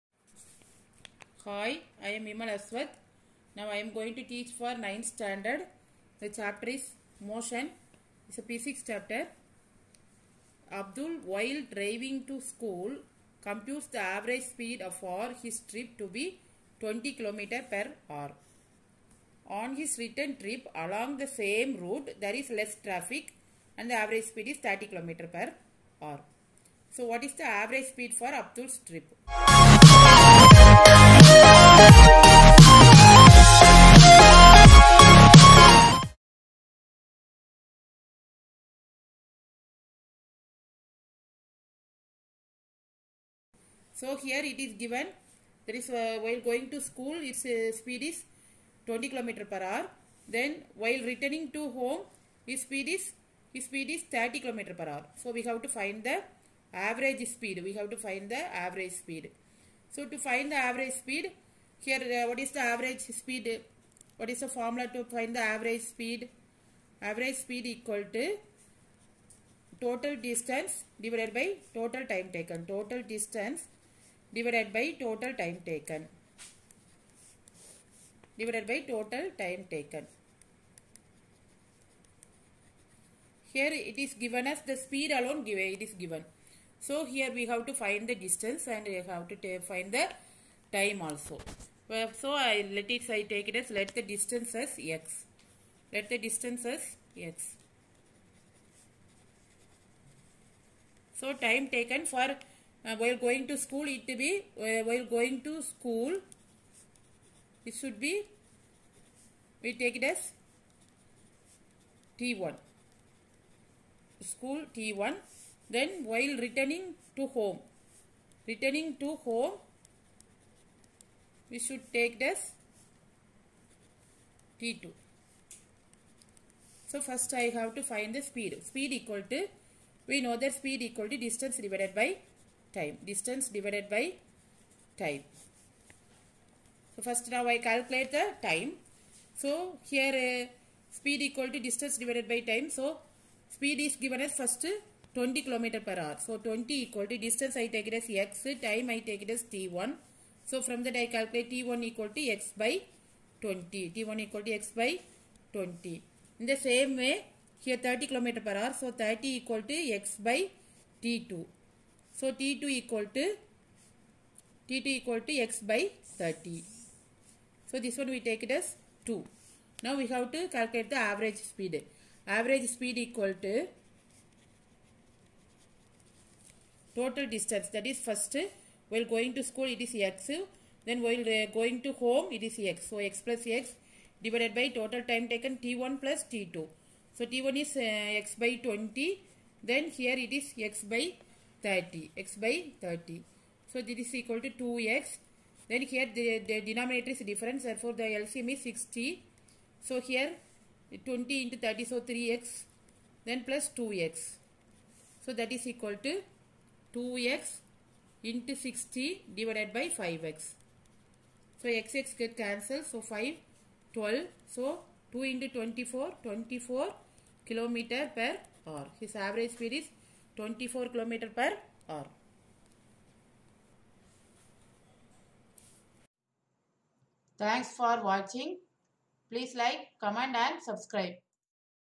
Abdul, while driving to school, computes the average speed for his trip to be 20 km/h.
yeah hope you enjoy this video
i am uploading both studies video,tech and entertaning videos
Abdul, while driving to school,# computes the average speed for his trip to be 20 km/h. On his return trip along the same route, there is less traffic and the average speed is 30 km/h. What is the average speed for Abdul's trip?